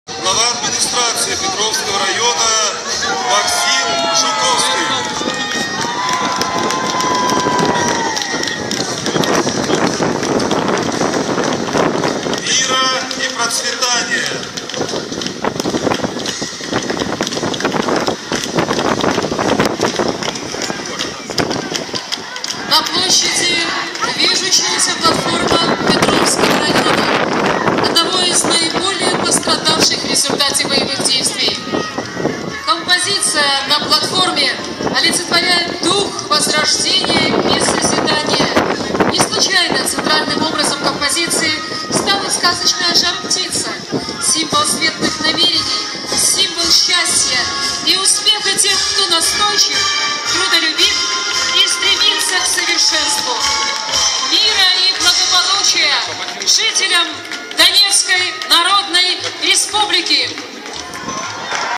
день рождения республики ДНР идёт Пролетарский район 2016_05_11
парад в честь дня образования Донецкой Народной Республики